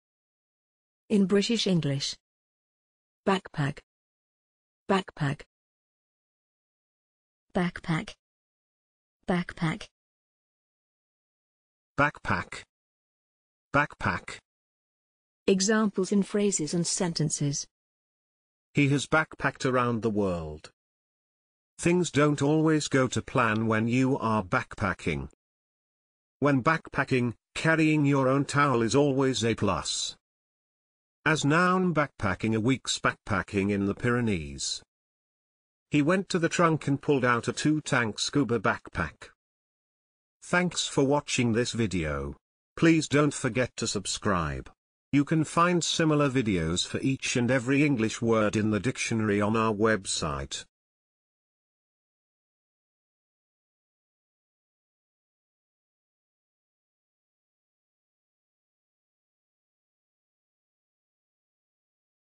backpack - pronunciation + Examples in sentences and phrases
This is the pronunciation of backpack in British English + a few examples of its pronunciation in phrases and sentences.